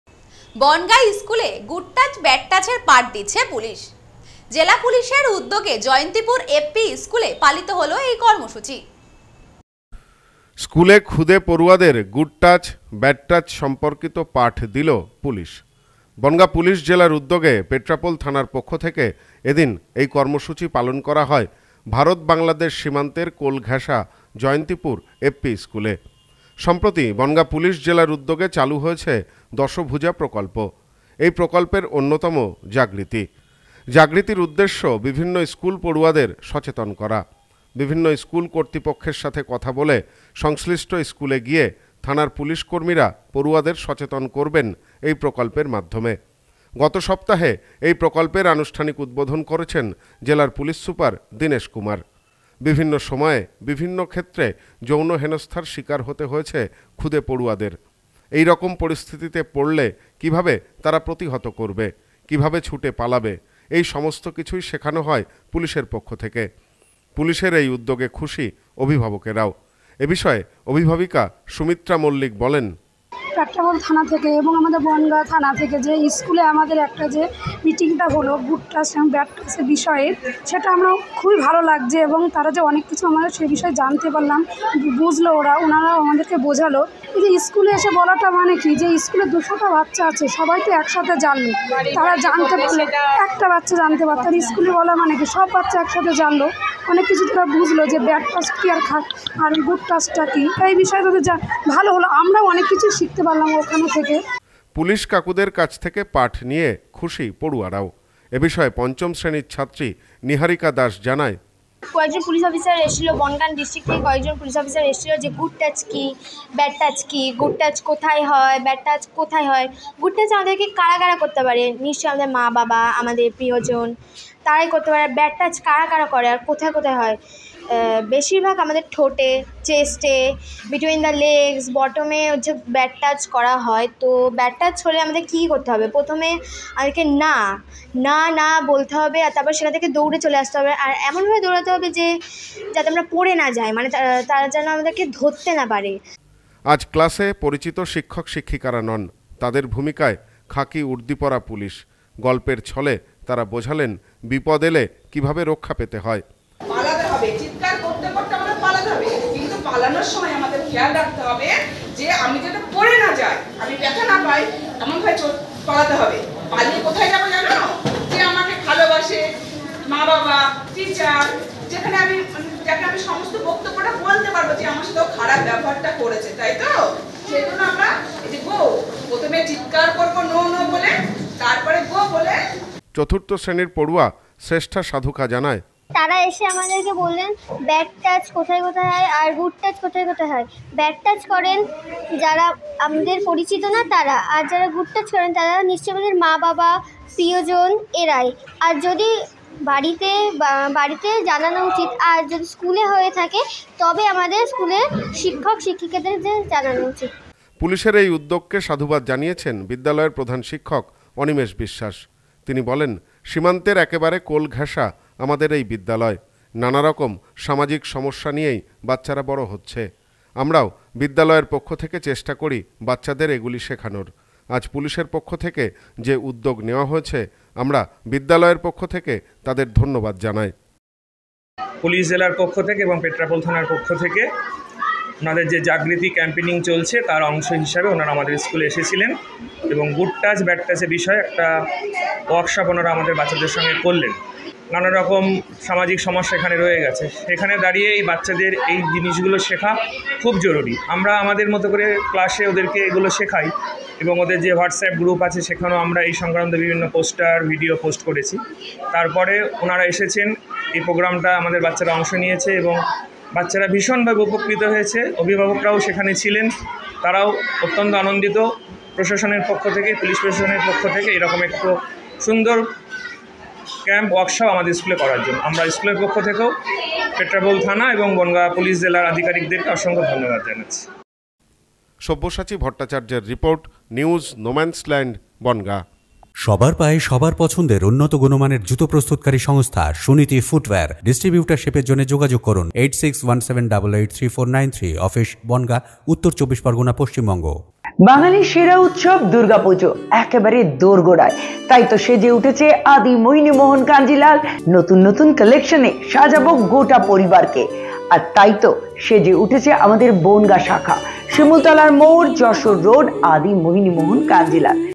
বনগাঁয় স্কুলে "গুড টাচ ব্যাগ টাচ" এর পাঠ দিচ্ছে পুলিশ।
বনগাঁয় স্কুলে "গুড টাচ ব্যাগ টাচ" এর পাঠ দিচ্ছে পুলিশ। জেলা পুলিশের উদ্যোগে জয়ন্তীপুর এফ.পি স্কুলে পালিত হল কর্মসূচি।